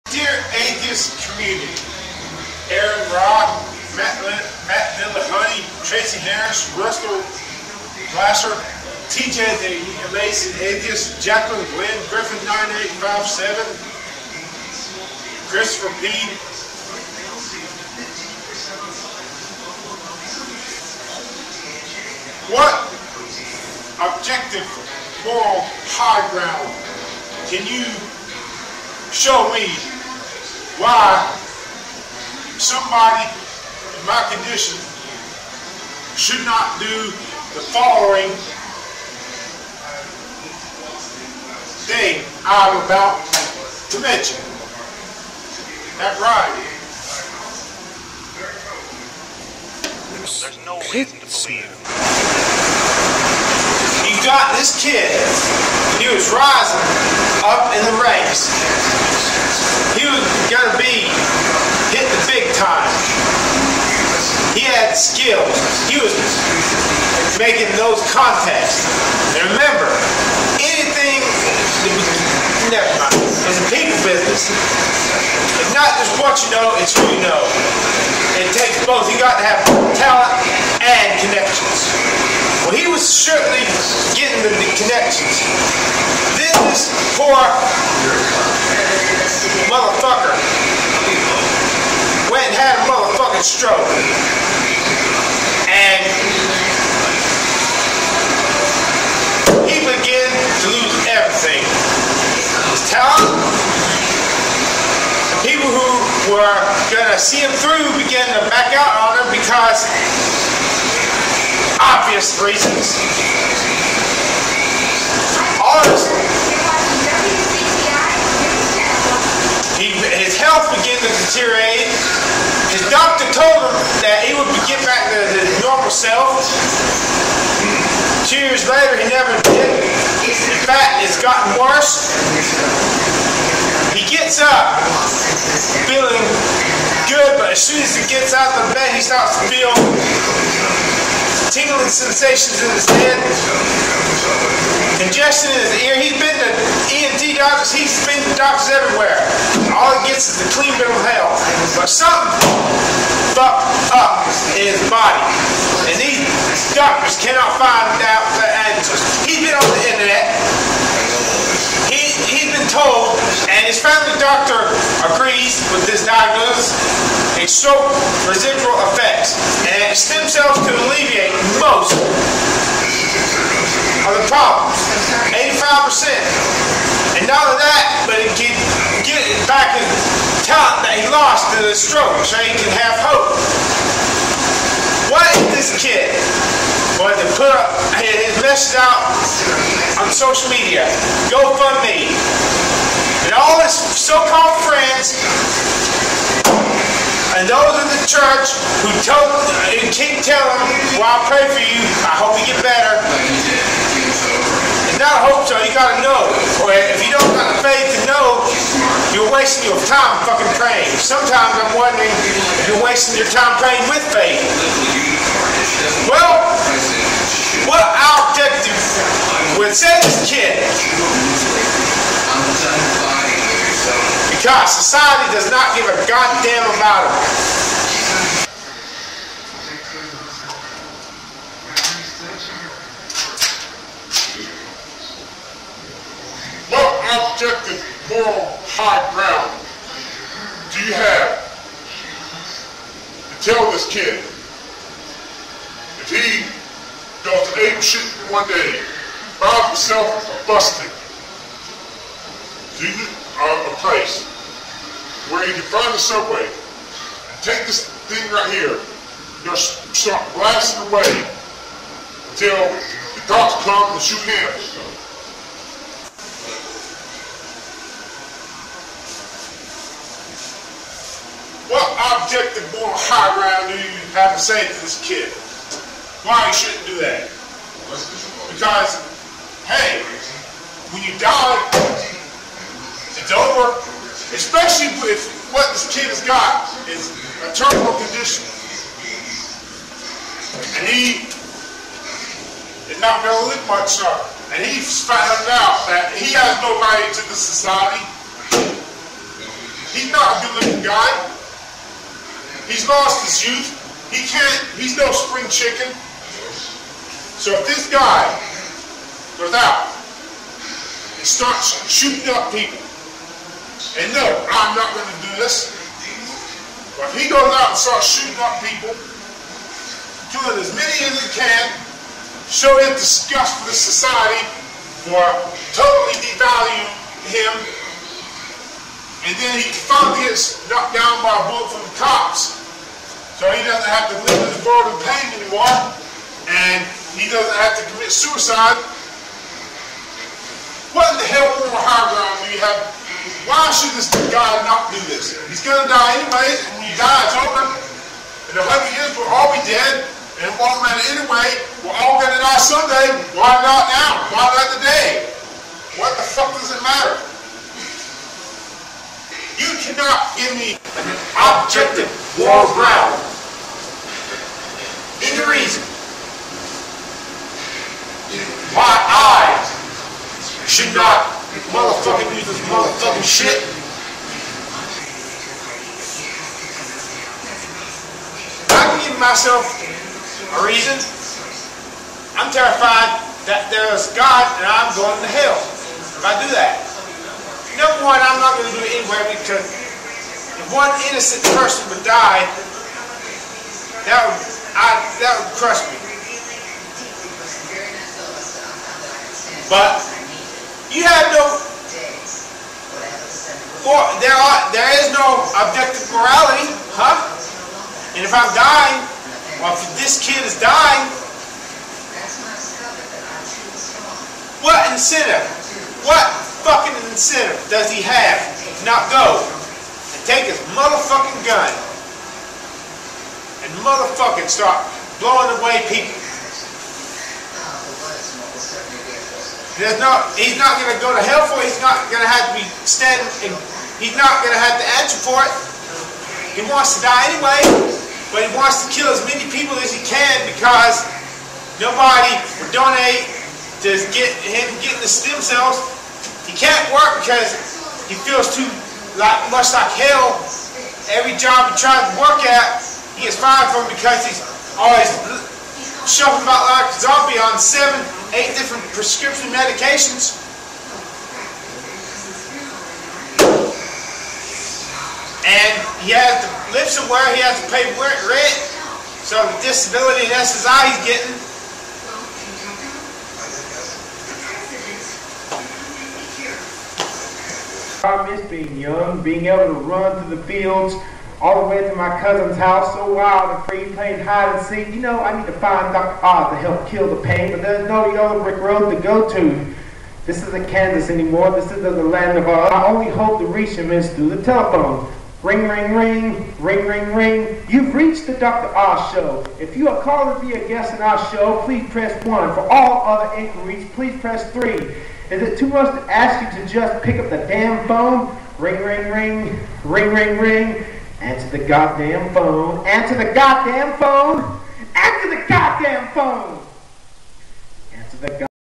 ATHEISTS, if there's NO GOD, what's OBJECTIVELY WRONG with a dying man committing MASS-MURDER?
Demon possessed guy committing suicide trying to kill HIS INNER DEMON!!!

The fucking demon's name is: STROKE, fucking stroke!

This man is NOT in his right mind, so he is not responsible or the fucking shit and filthy and rivers of raw fucking sewage rolling like a tidal wave outta his fucking mouth!